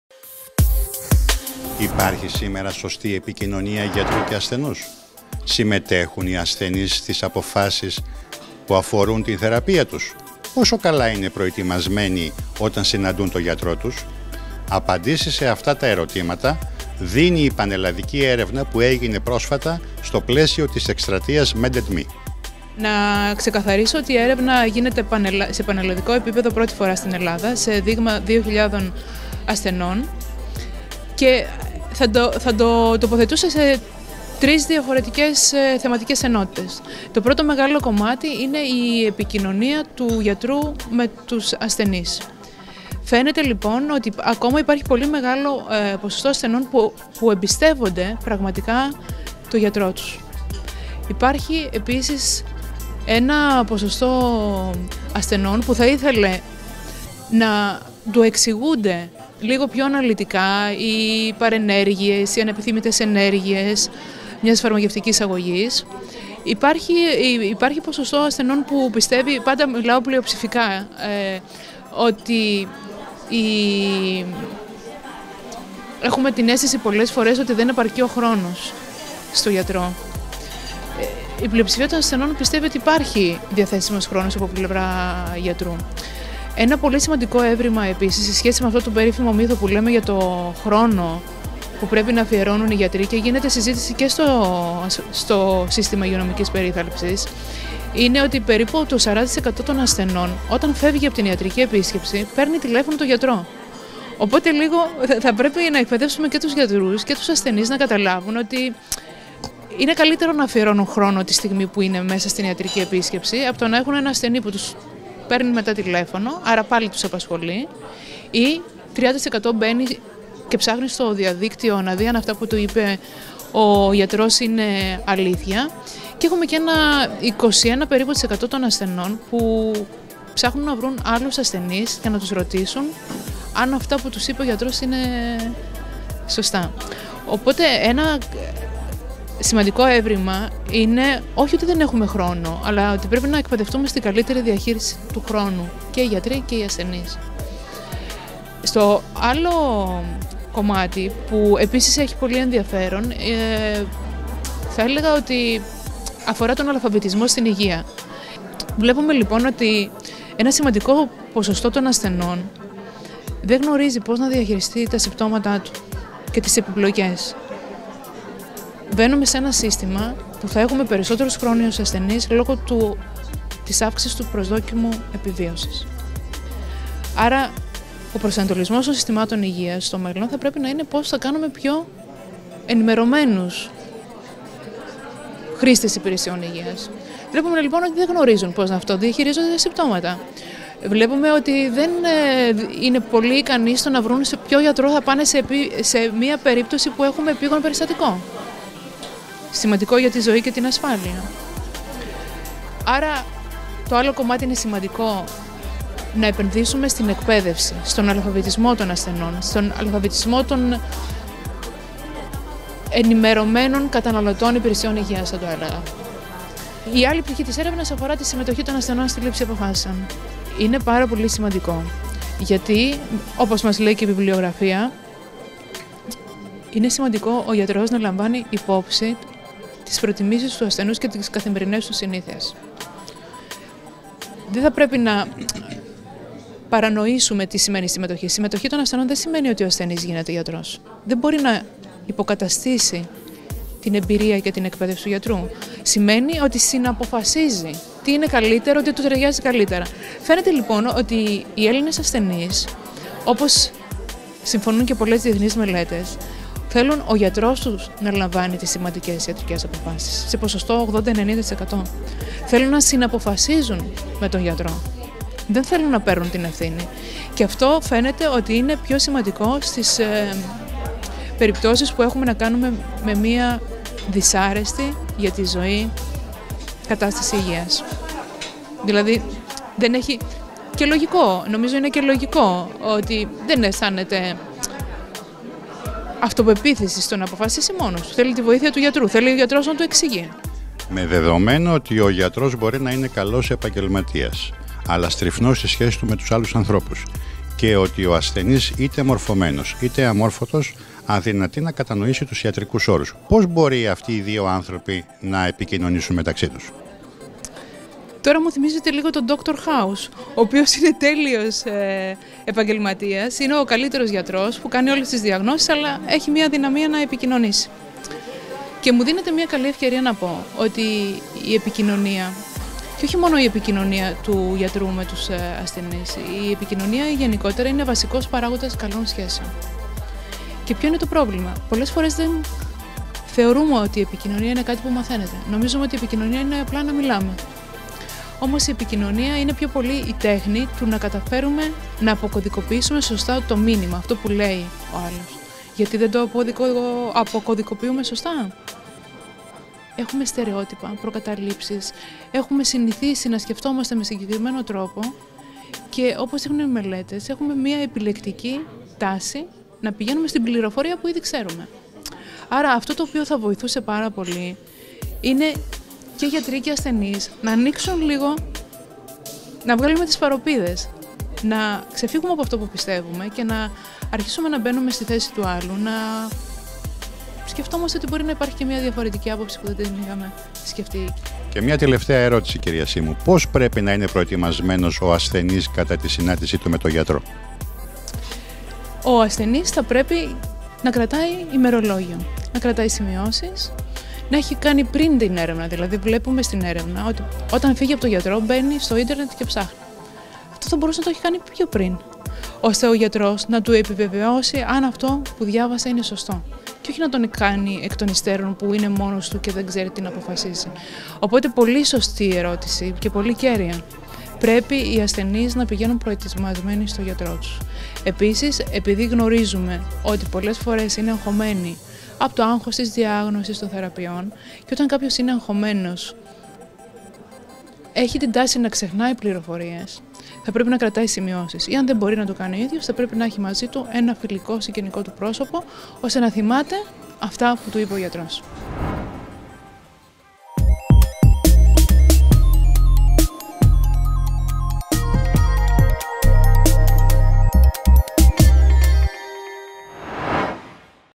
Πανελλαδική έρευνα για την επικοινωνία Γιατρού-Ασθενούς
Πανελλαδική έρευνα για την «Επικοινωνία Γιατρού – Ασθενούς,  τον Αλφαβητισμό στην Υγεία και τη Συμμετοχή των Ασθενών στη Λήψη Αποφάσεων»